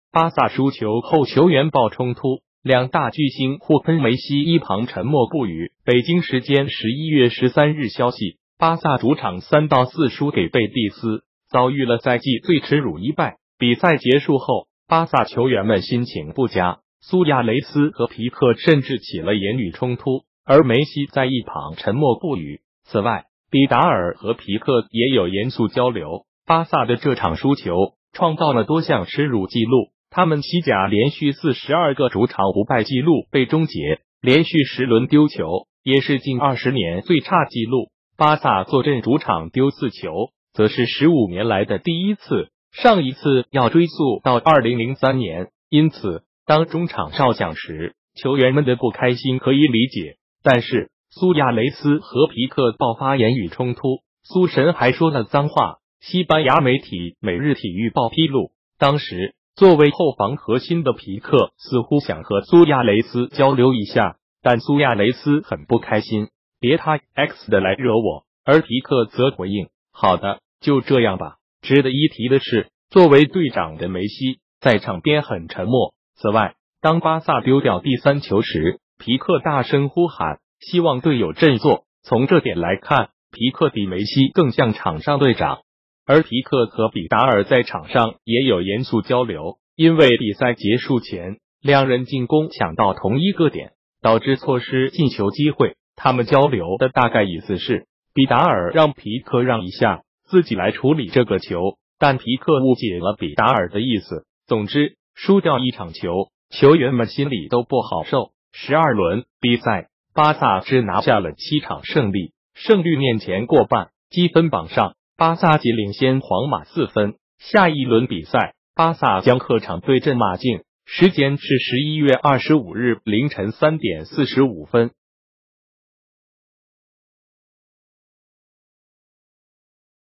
巴薩輸球后球員爆沖突！兩大巨星互噴 梅西一旁沉默不語
巴薩主場3-4輸給貝蒂斯，遭遇了賽季最恥辱一敗。比賽結束后，巴薩球員們心情不佳，蘇亞雷斯和皮克甚至起了言語沖突，而梅西在一旁沉默不語。